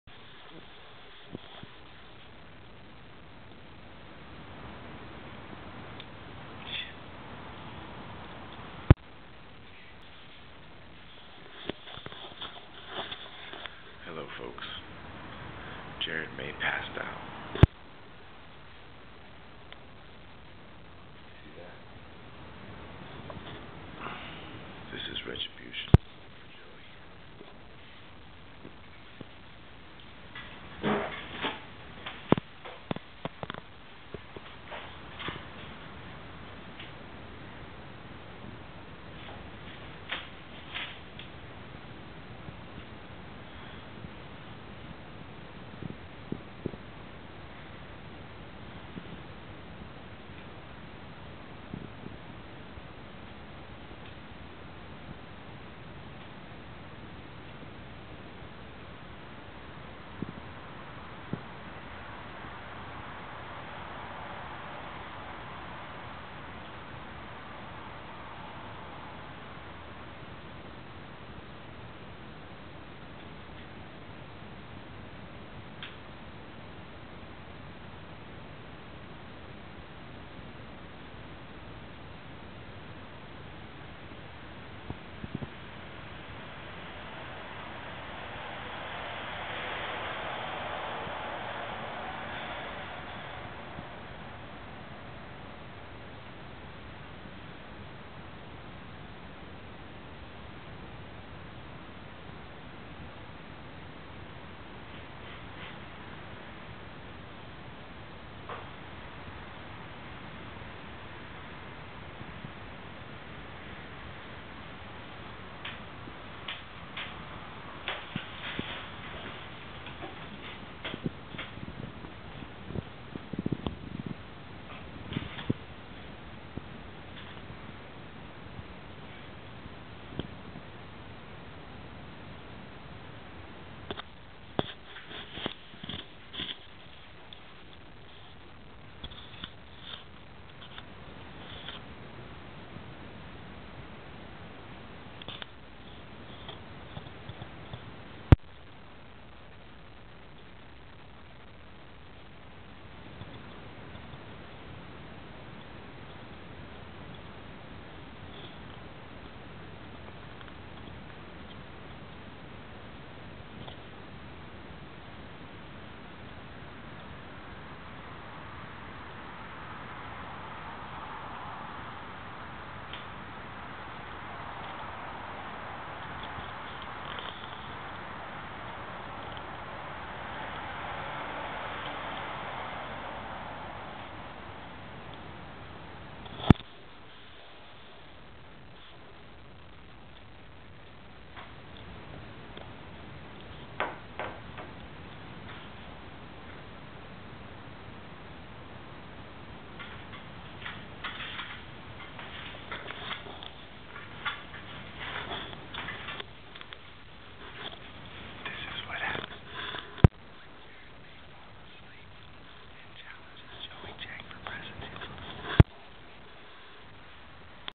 terry trims jared's mustache
drunken beer drinkin antics at the end of the pleasant revolution's northwest tour from august 2009...